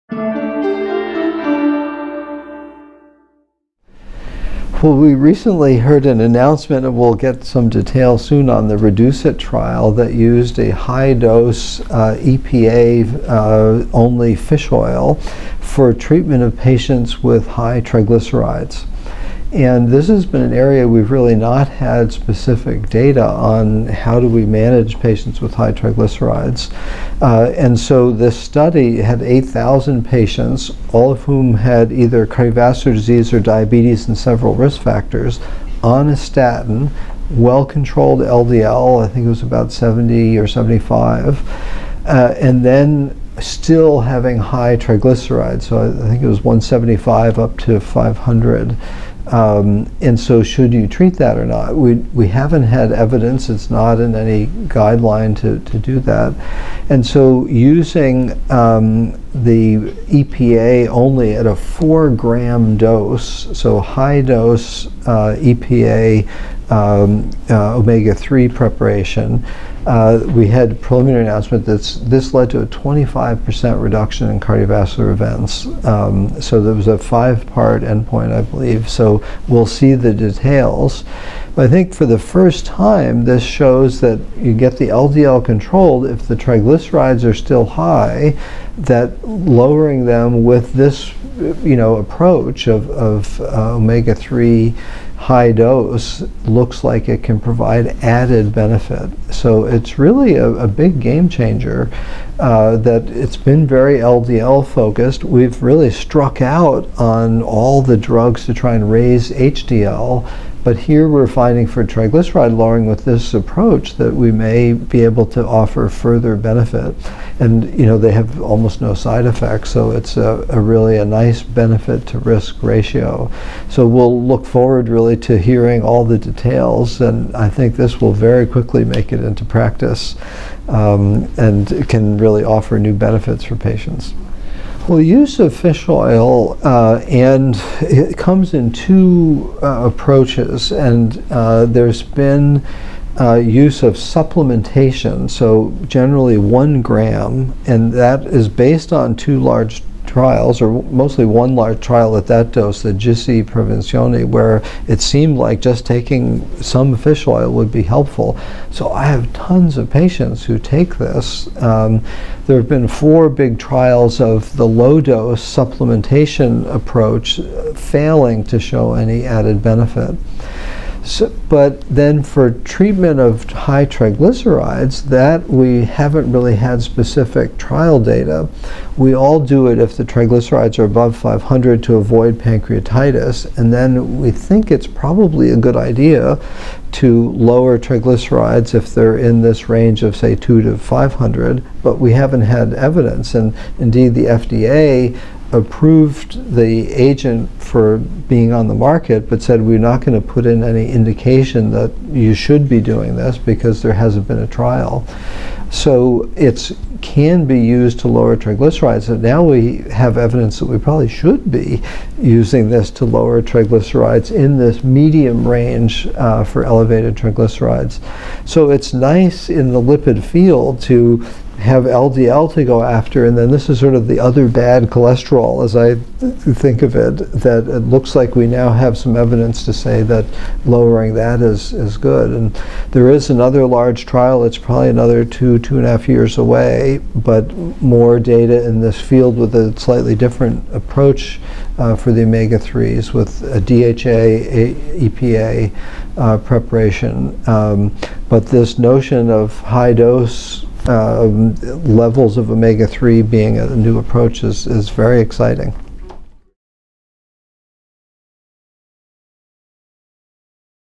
Vascepa: Current Uses and Emerging Results from REDUCE-IT
Christopher Cannon, MD, Professor of Medicine, Harvard Medical School, discusses the recent topline results from the Vascepa cardiovascular (CV) outcomes trial, REDUCE-IT, a global study of 8,179 statin-treated adults with elevated CV risk. According to a recent announcement from Amarin, REDUCE-IT met its primary endpoint demonstrating a 25% relative risk reduction, to a high degree of statistical significance, in major adverse CV events (MACE) in the intent-to-treat patient population with use of Vascepa 4 grams/day as compared to placebo.

Amarin Corp is the Manufacturer of Vascepa